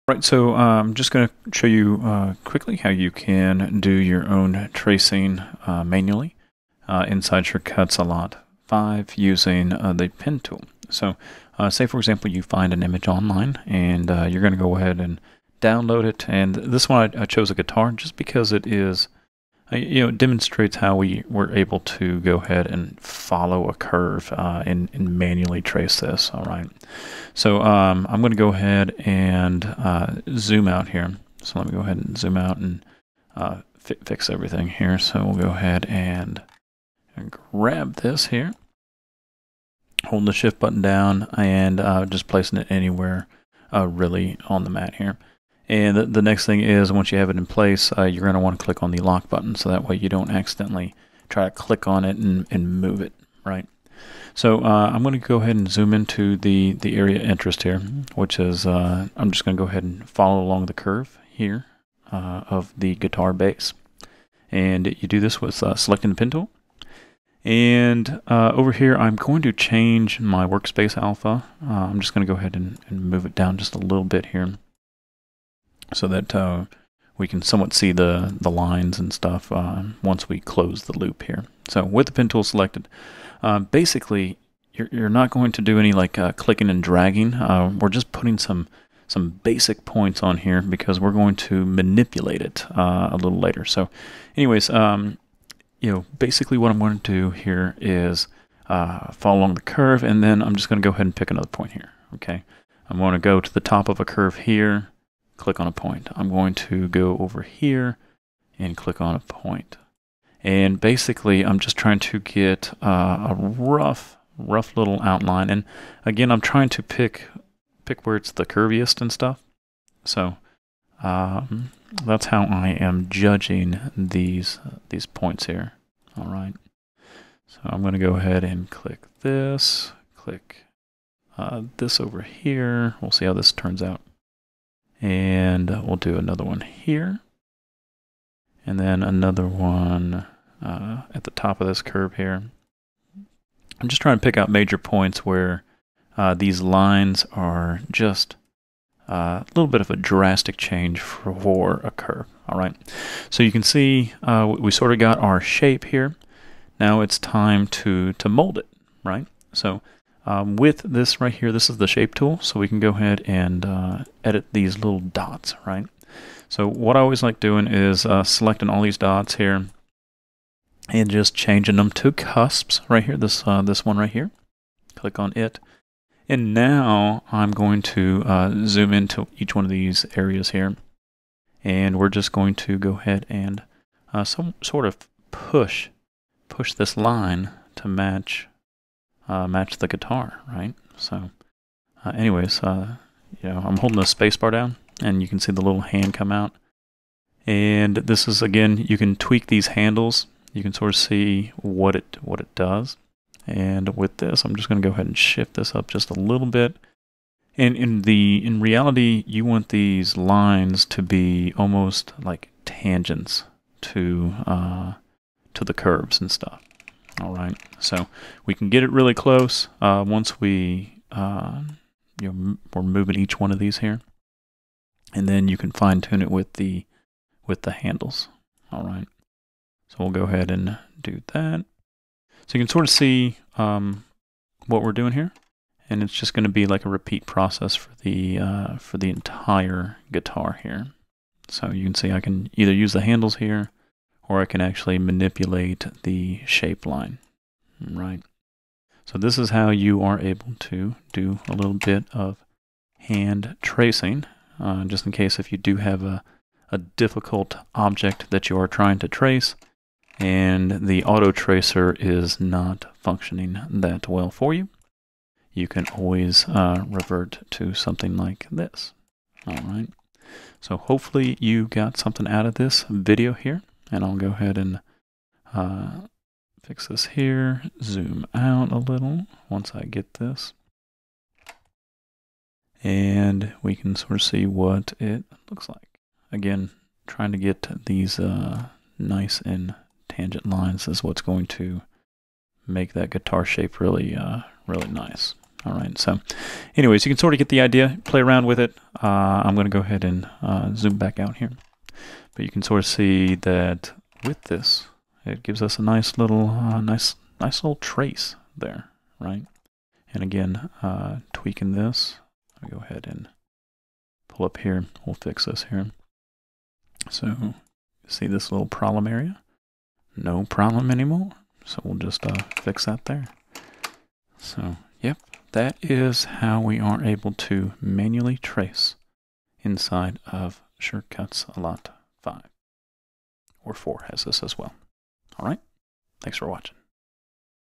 Sure Cuts A Lot 5 - Manual Tracing with Pen Tool (a little advanced)
New Purchase of SCAL 5